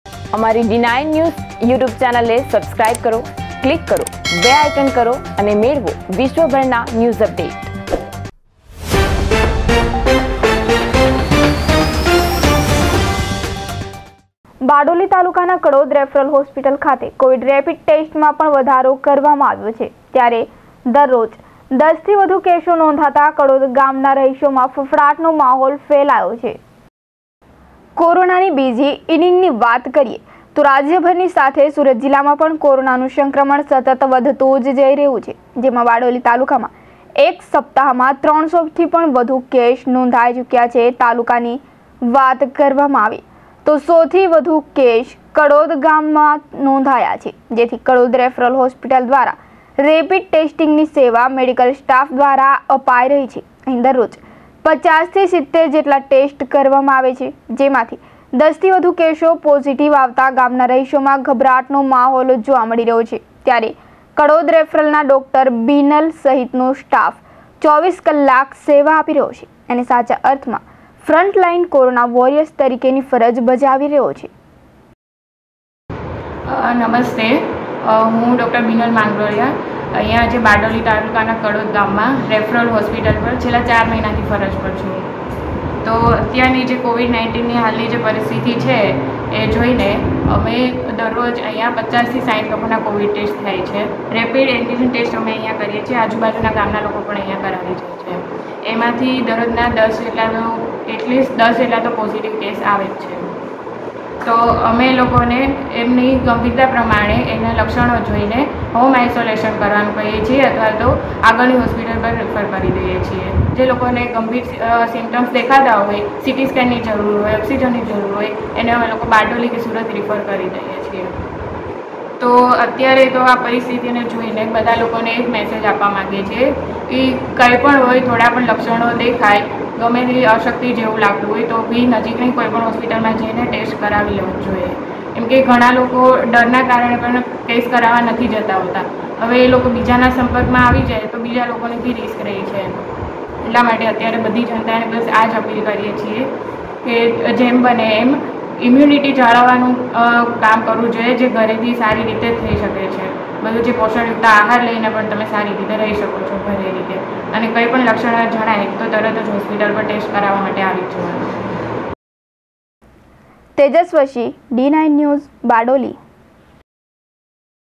D9NEWS : બારડોલી તાલુકાના કડોદ રેફરલ હોસ્પિટલ ખાતે કોવિડ રેપીડ ટેસ્ટમાં વધારો
Title:  બારડોલી તાલુકાના કડોદ રેફરલ હોસ્પિટલ ખાતે કોવિડ રેપીડ ટેસ્ટમાં વધારો
Name of the Reporter : Tejash vashi
Date & Time : 13/04/2021 12:00:00

Description
બારડોલી તાલુકાના કડોદ રેફરલ હોસ્પિટલ ખાતે કોવિડ રેપીડ ટેસ્ટમાં વધારો કરવામાં આવ્યો છે ત્યારે દરરોજ ૧૦ થી વધુ કેસો નોંધાતા  કડોદ ગામના રહીશોમાં ફફડાટનો માહોલ જોવા મળી રહ્યો છે..

વિઓ : ૧  કોરોનાની બીજી ઇનિંગની વાત કારીએતો રાજ્યભરની સાથે સુરત જિલ્લામાં પણ કોરોનાનું સંક્રમણ સતત વધી રહ્યું છે. જેમાં બારડોલી તાલુકામાં એક સાપ્તાહમાં 300 થી પણ વધુ કેસો નોંધાયા છે. તાલુકાની વાત કારીએતો સૌથી વધુ કેસ કડોદ ગામમાં નોંધાતા કડોદ રેફરલ હોસ્પિટલ દ્વારા રેપીડ ટેસ્ટિંગની સેવા મેડિકલ સ્ટાફ દ્વારા અપાય રહી છે. અહીં દરરોજ ૫૦ થી ૭૦ જેટલા ટેસ્ટ કરવામાં આવે છે તેમાંથી ૧૦ થી વધુ કેસો પોઝિટિવ આવતા ગામના રહીશોમાં ગભરાટનો માહોલ જોવા મળી રહ્યો છે ત્યારે કડોદ રેફરલ ના ડો. બિનલ સહિતનો સ્ટાફ ૨૪ કલાક સેવા આપી સાચા અર્થમાં ફ્રન્ટ લાઈન કોરોના વોરિયર્સ તરીકેની ફરજ બજાવી રહ્યા છે.